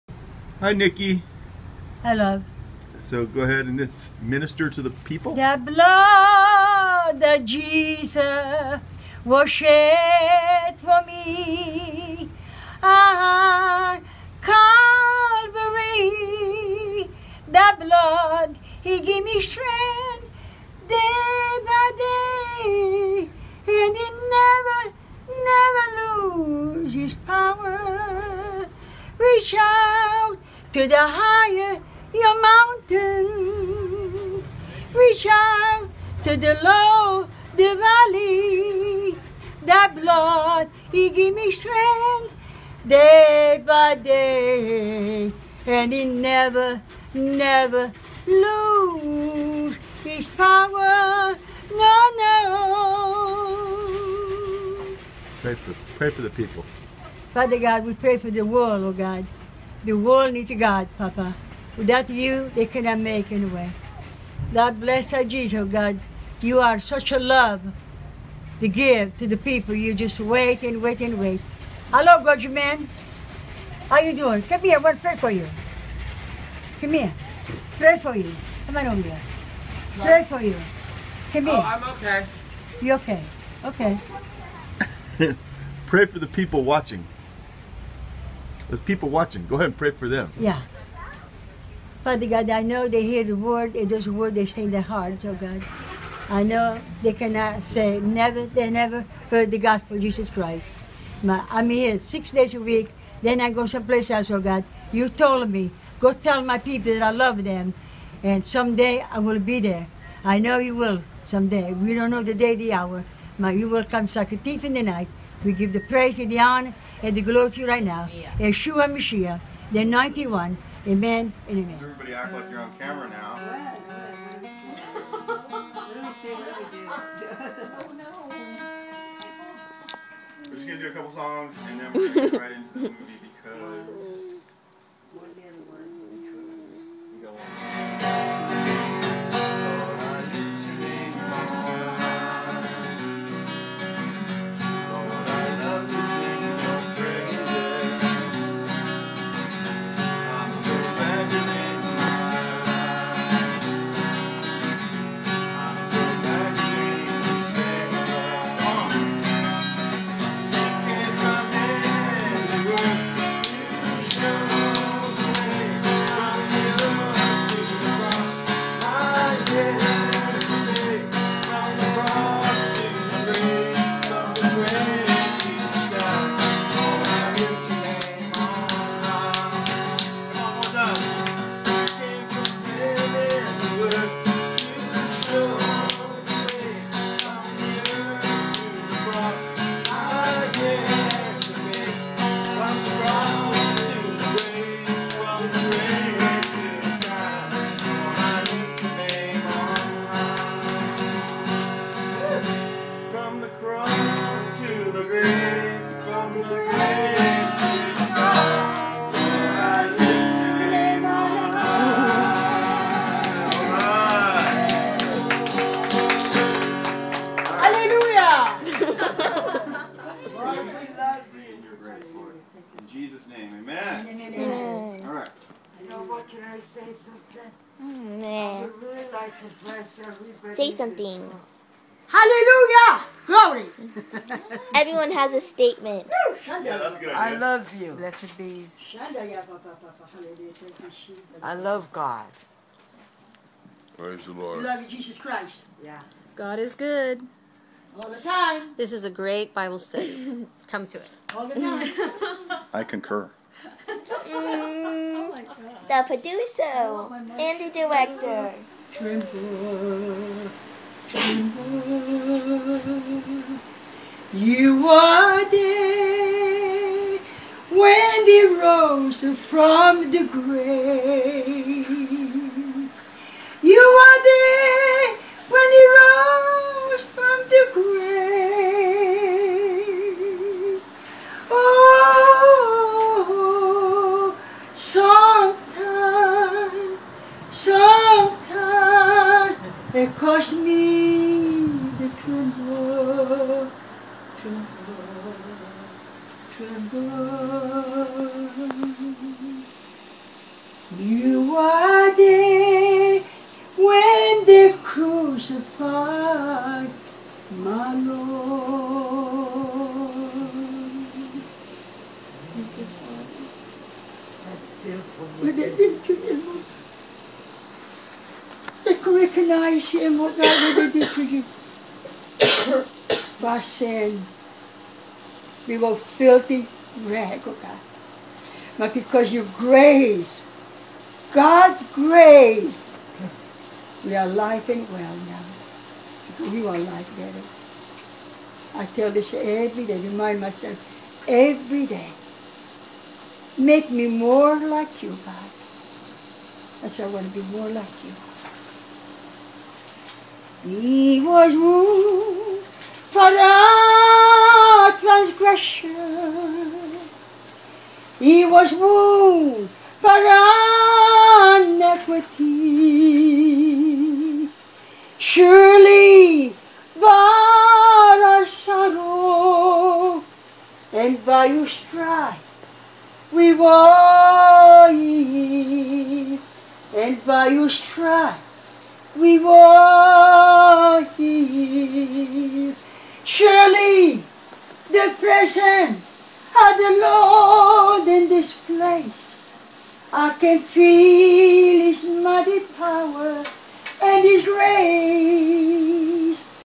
Nicky Tedesco - South Bay Evangelist
Nicky Tedesco was well known all over the South Bay (Hermosa, Redondo, and Torrance, CA) for ministering salvation and healing with a strong anointing and glory of God in her life. People all over wanted her to lay hands and pray because there were always fresh baptisms of the Holy Spirit. She had an incredible feeding ministry to the poor, and a huge Thanksgiving feeding at Hermosa beach that she started. I was honored to minister with Nicky at my homegroup and on weekends feeding the homeless for many years. Play the clip and let her sing over you and even now you will feel the Presence of the Holy Spirit. She went to Heaven in her mid 80s.